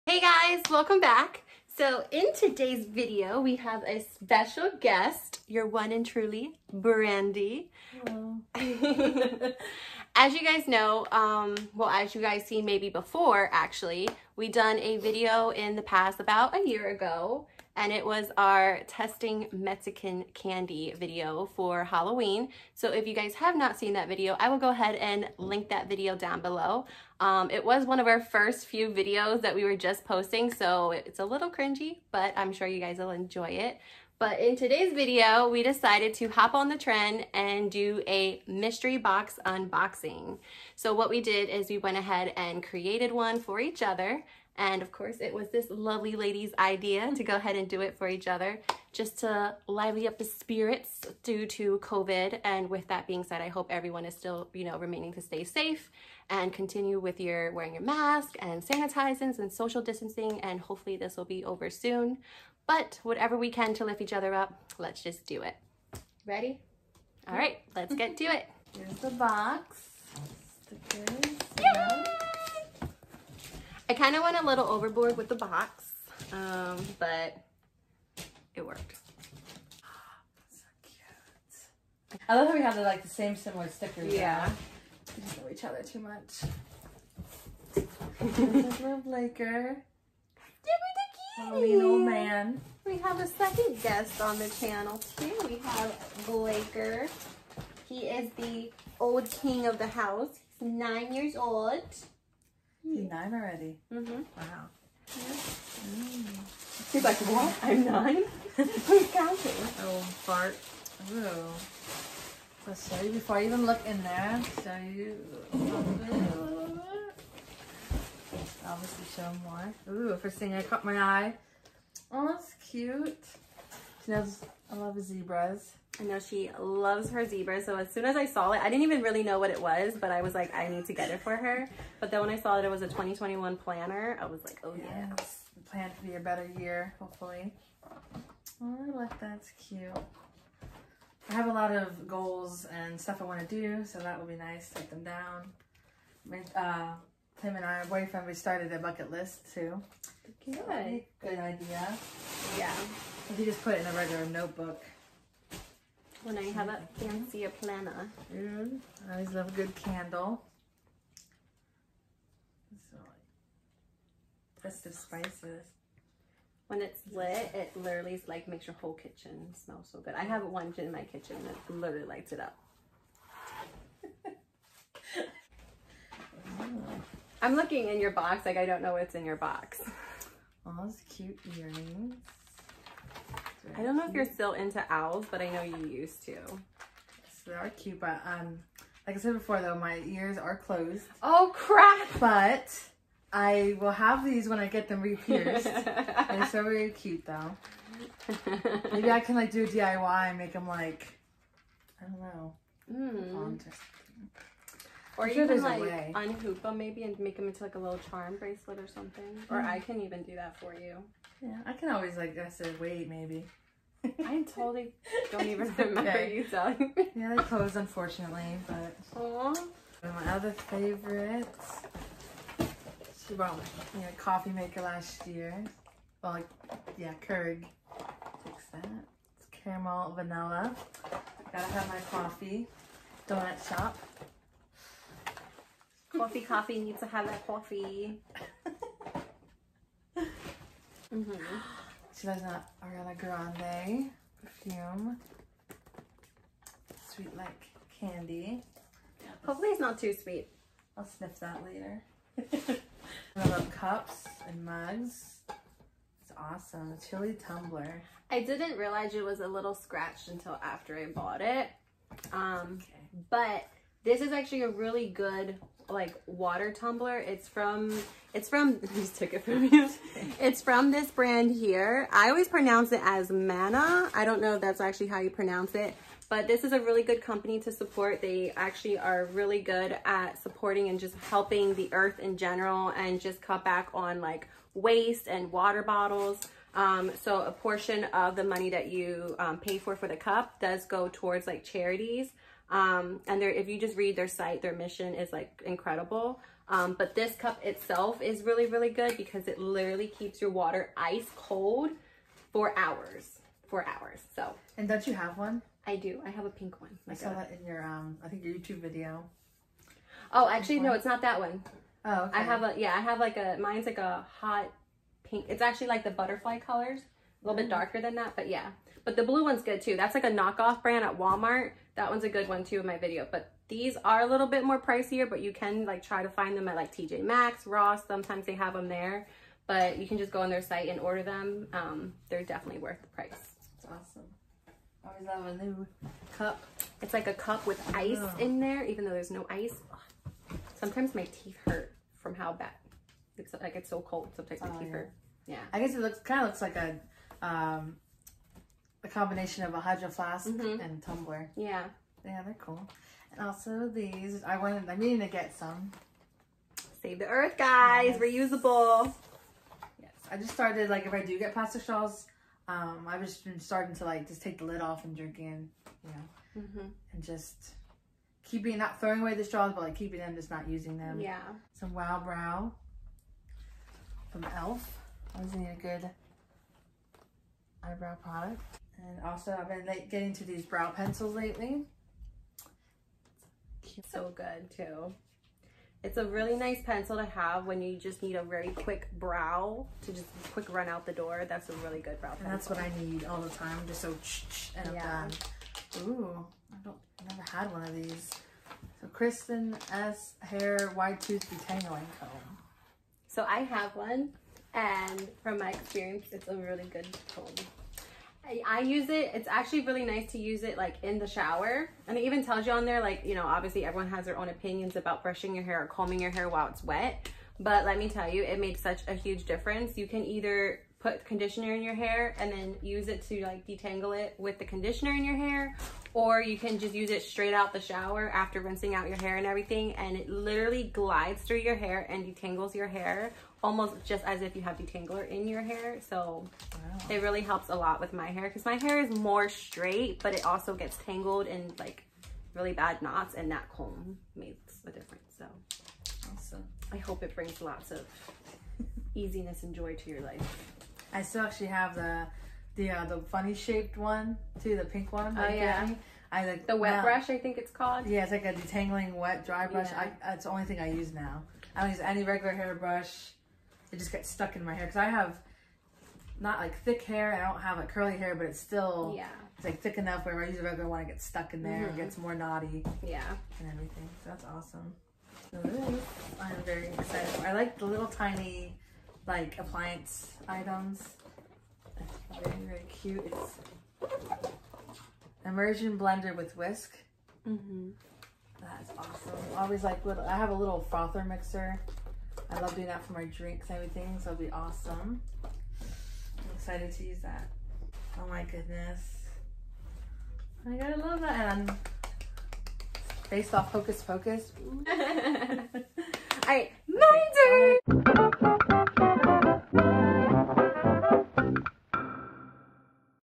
Mystery Box Unboxing| Part 1
Hey everyone!

I’m this video my best friend Brandie and myself decided to create a mystery box for one another to add some joy to each other’s lives. This was so much fun! Part 1 is Brandie unboxing the box I created for her. Part 2 of me unboxing the box she created will be uploaded separately to prevent a super long video haha